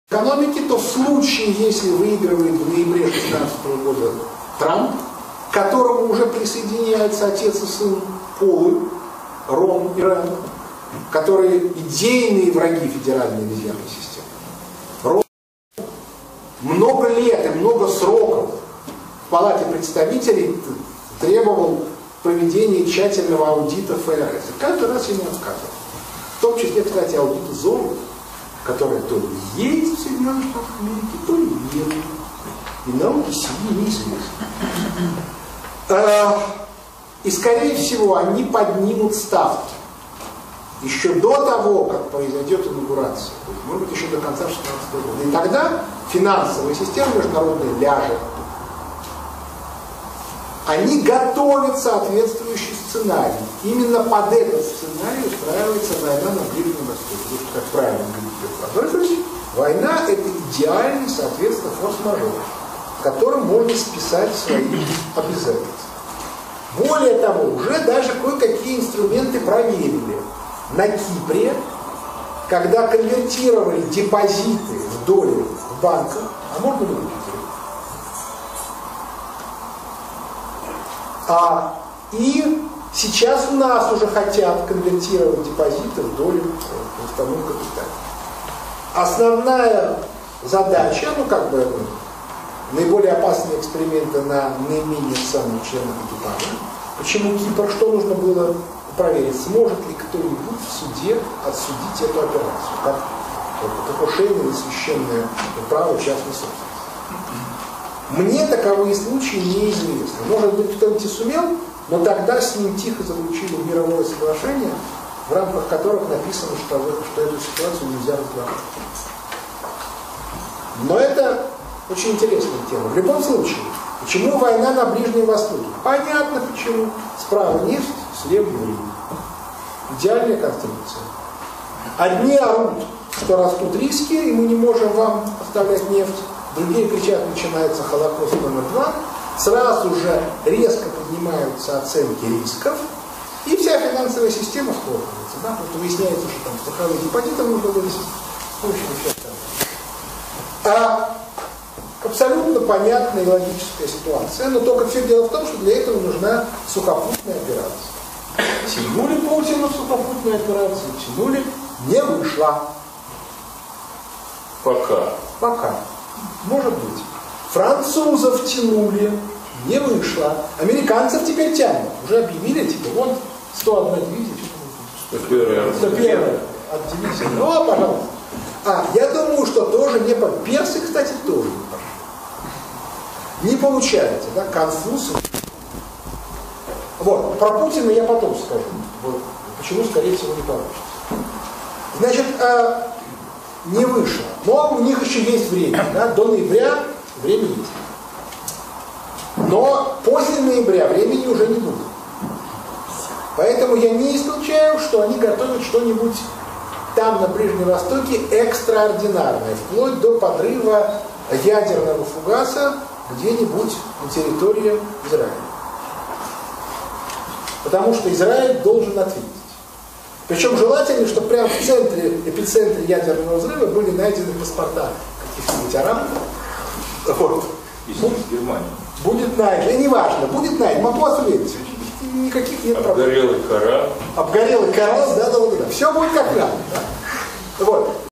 04.02.2016 Хазин ожидает подрыв ядерного фугаса в Израиле в течении года
Скорее всего поднятие учётной ставки ФРС США произойдёт ещё до инаугурации нового президента. Тогда мировая финансовая система ляжет. Под это дело организуется война на Ближнем Востоке. Там слева нефть, справа евреи. Война - это идеальный форс-мажор, под который можно списать свои обязательства.

Одни будут орать, растут риски, поэтому мы не может качать вам нефть, вторые, что начинается Холокост номер 2...Вся мировая финансовая система схлопывается...

Хазин не исключает, что на Ближнем Востоке готовится, что-то экстра-ординарное, вплоть до подрыва ядерного фугаса в Израиле. Потому что Израиль должен ответить.